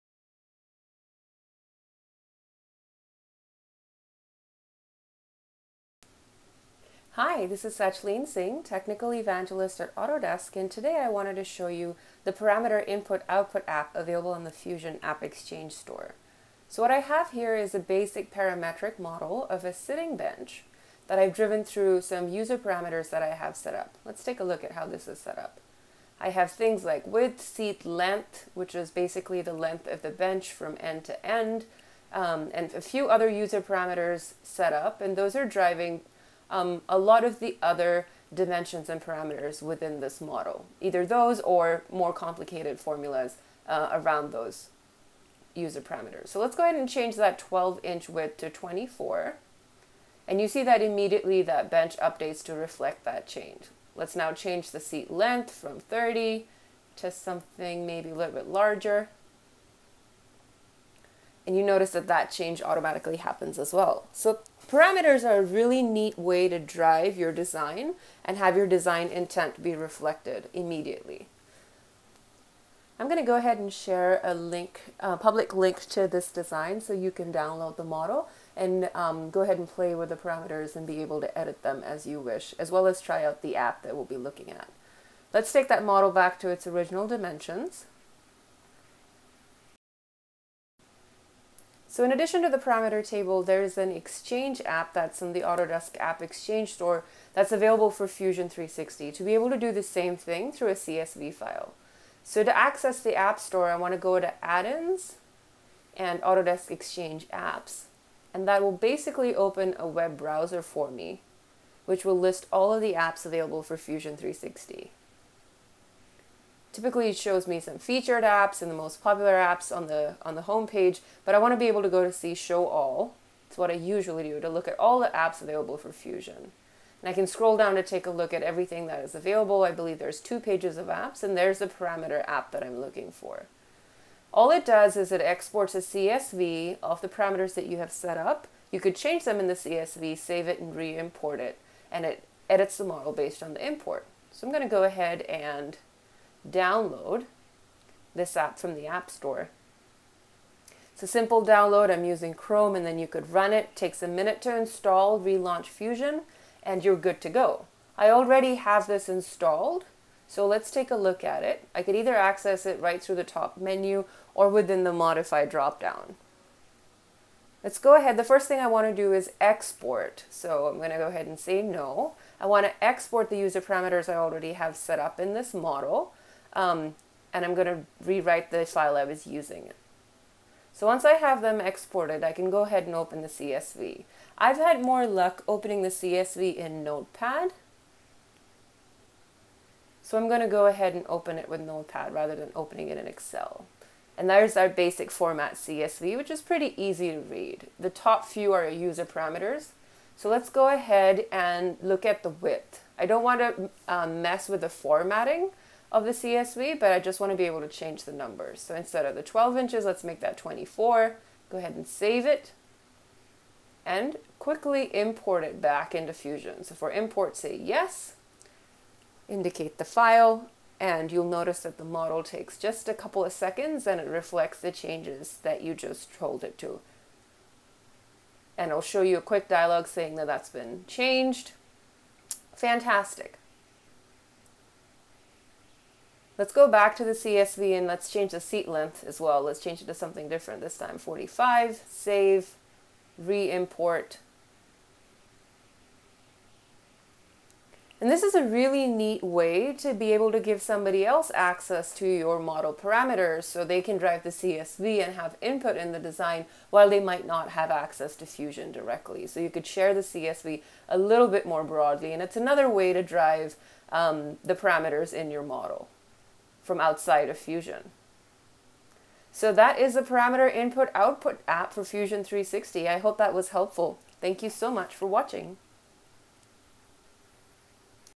QUICK TIP: Using Parameters and the Parameter I/O App
Ever wonder how you can drive a Fusion 360 design with a few key Parameters? Fusion 360 Evangelist, Sachlene Singh (@Sachlene_Singh) goes through how she set up a parametric model of a bench. She also shows the Parameter I/O app - available on the App Exchange Store.

Thanks for April Wilkerson for the design inspiration!